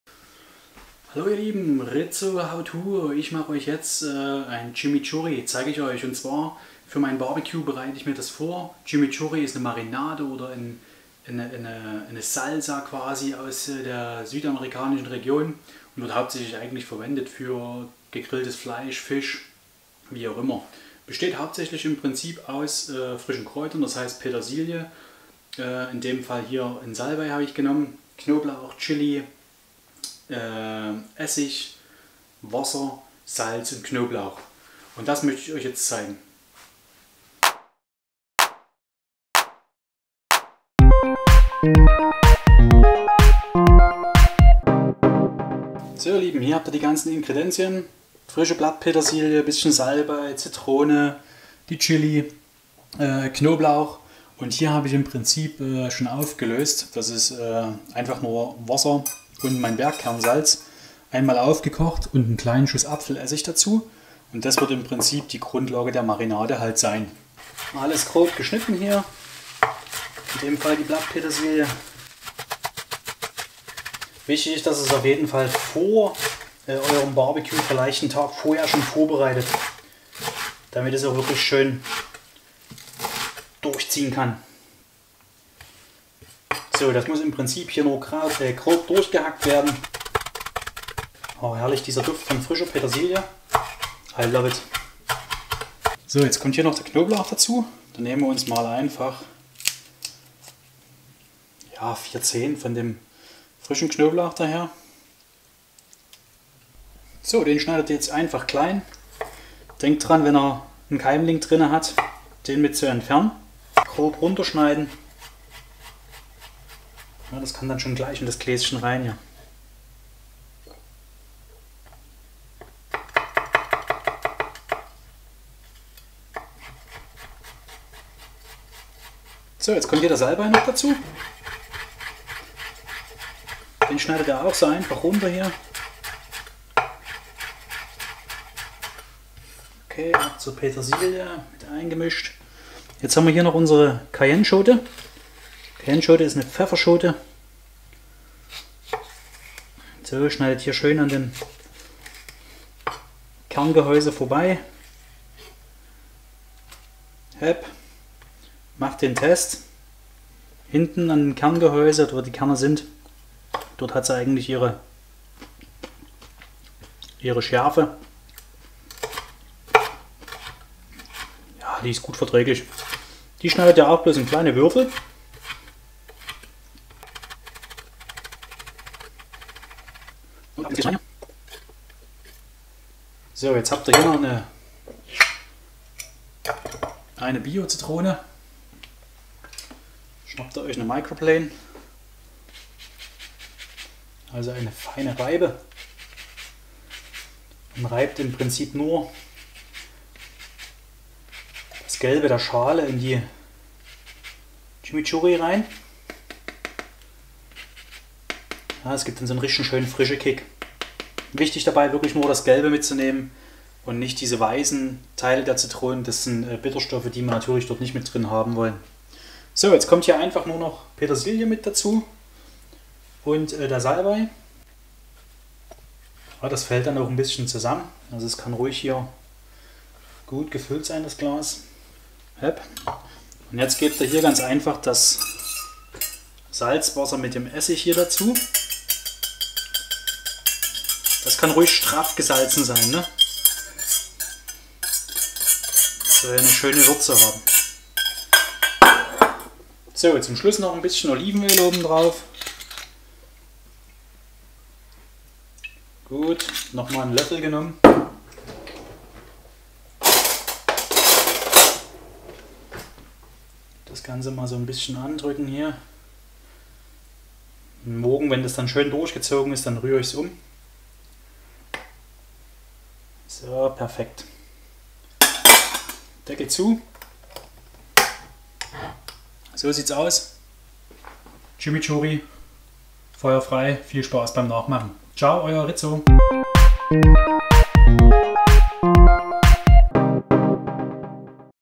Rizzos Howto Chimichurri
Was gehört in ein echtes argentinisches Chimichurri Rezept? In diesem Howto zeige ich dir wie du Chimichurri selber machen kannst.